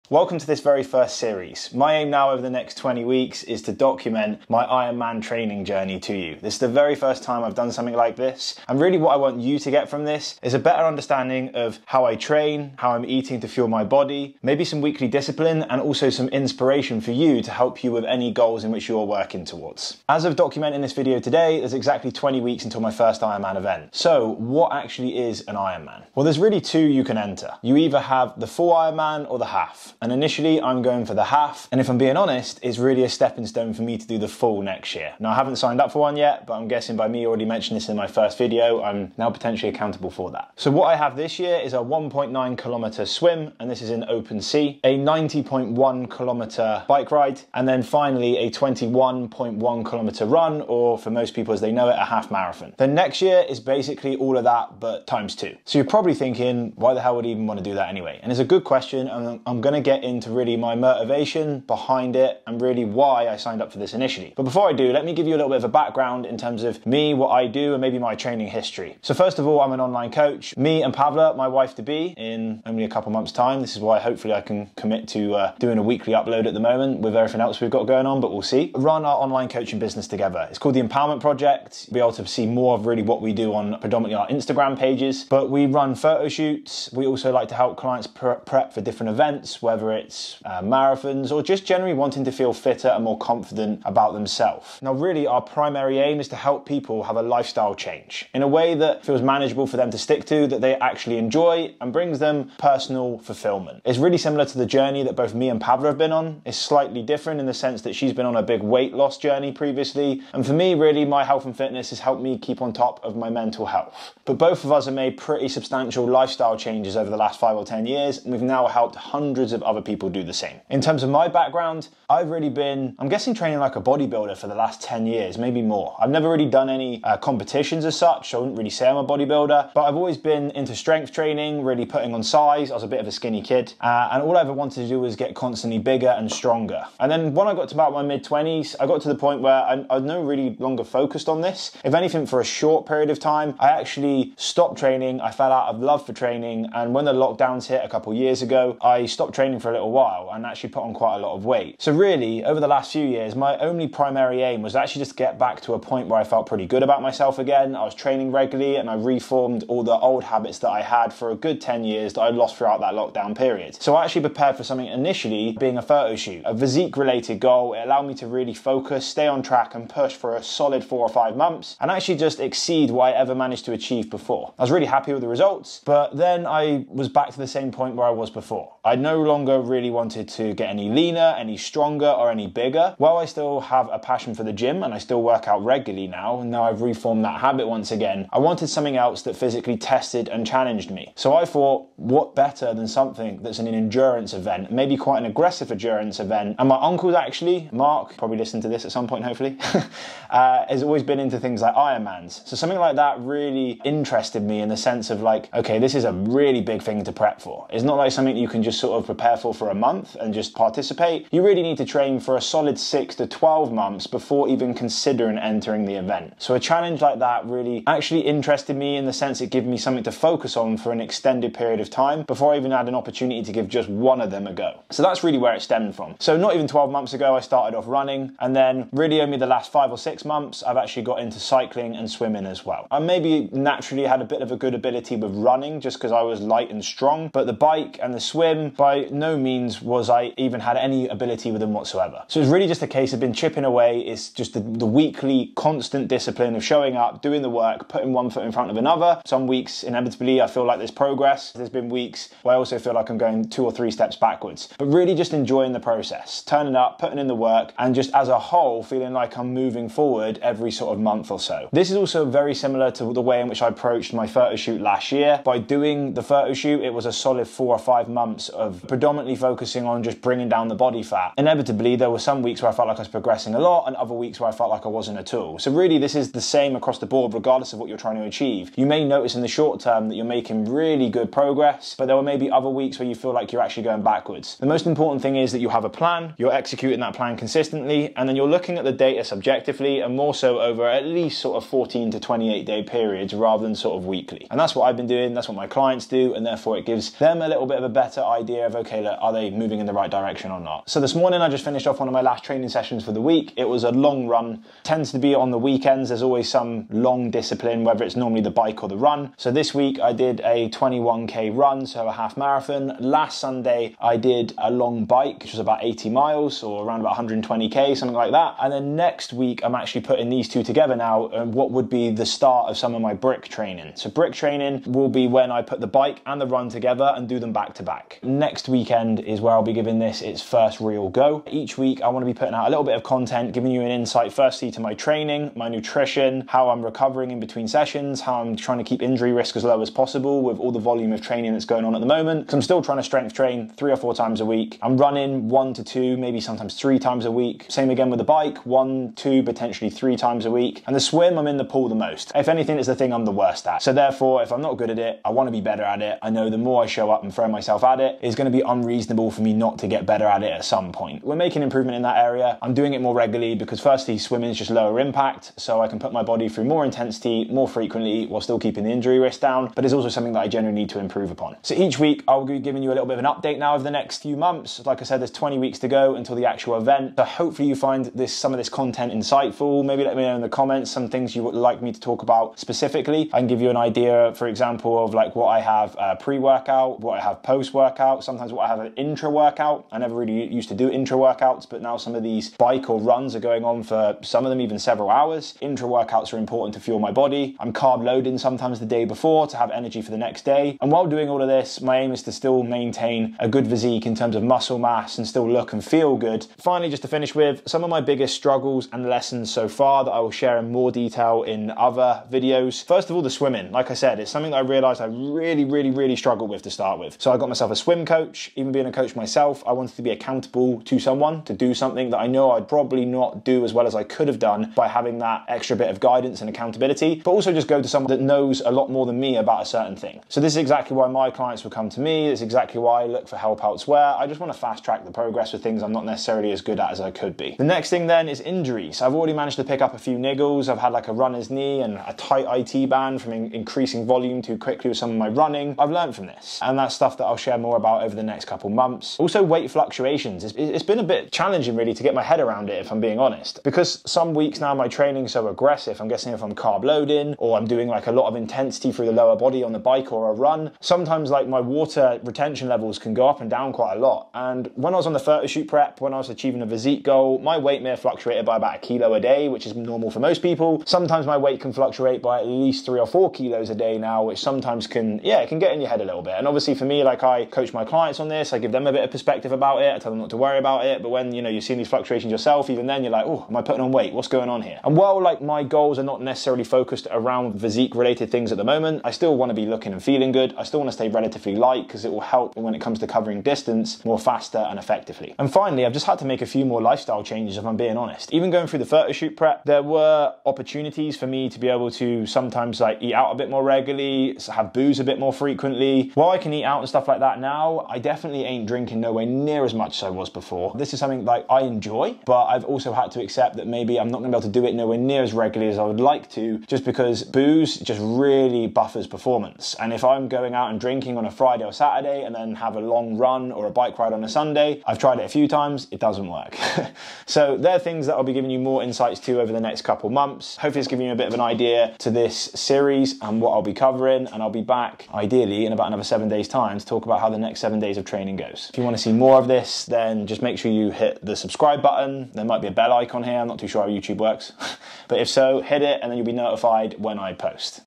Bodybuilding to ironman triathlons - 20 week countdown
I've been weight training for over 10 years & this year decided to challenge myself with an ironman triathlon (Half distance)

over the next 20 weeks I'll be documenting my journey in the lead up to the lead up to the event in the hope to give you some value, discipline & inspiration with your goals

I'll share all things about my training, nutrition as well as highs & lows along the way